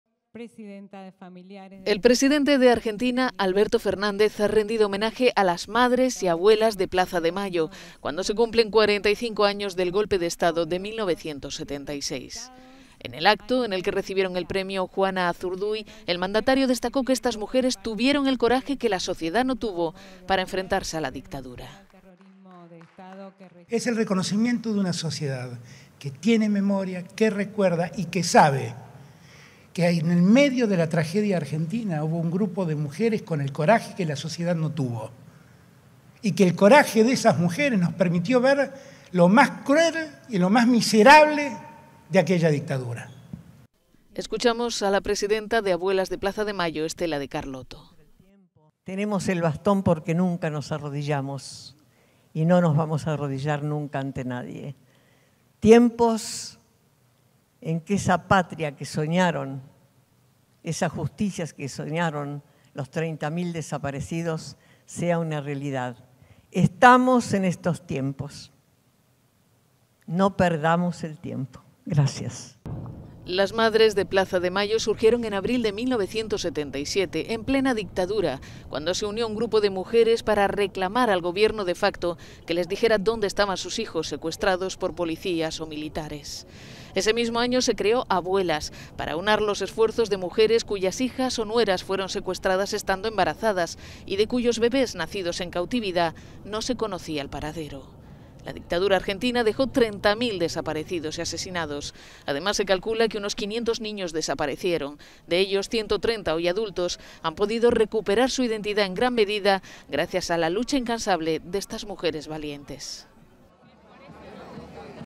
Argentina | Homenaje a la Madres y Abuelas de Plaza de Mayo
Argentina | Homenaje a las Madres y Abuelas de Plaza de Mayo, en el 45 aniversario del golpe de Estado. El presidente Alberto Fernández destacó que estas mujeres 'tuvieron el coraje que la sociedad no tuvo' para enfrentarse a la dictadura